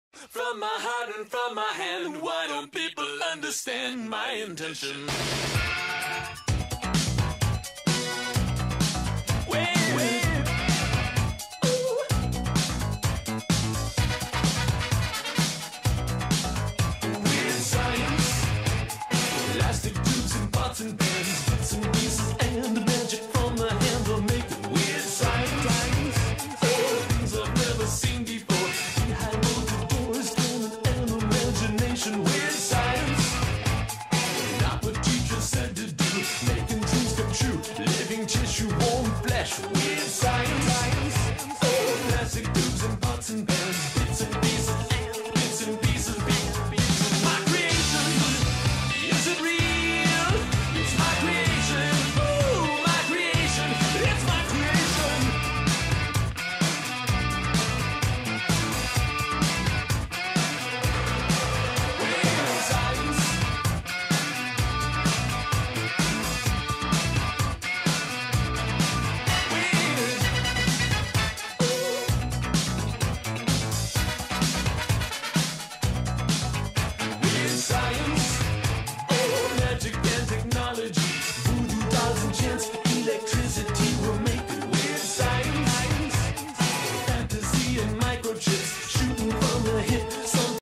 Fun Science Chromatography Art Project
This is a short video to help introduce a nice end of year activity on chromatography that students can carry out